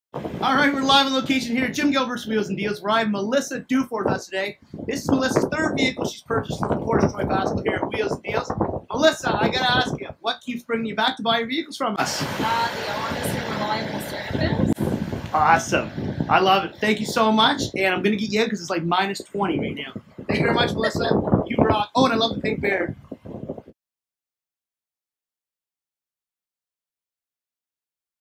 Melissa Dufour, 2016 Jeep Cherokee | JIM GILBERT'S WHEELS & DEALS
All right, we're live on location here at James Gilbert, Wheels and Deals, where I have Melissa Dufour with us today. It's Melissa's third vehicle she's purchased from Troy Fasquel here at wheels and Deals. Melissa, I got to ask you, what keeps bringing you back to buy vehicles from us? The honest and reliable service. Awesome. I love it. Thank you so much, and I'm going to get you out because it's like minus 20 right now. Thank you very much Melissa, you rock. Oh, and I love the pink bear.

Serving:
Oromocto
Fredericton
Moncton
Saint John
Woodstock

Used Vehicles for sale.

402 St Mary's St
Fredericton, NB
E3A 8H5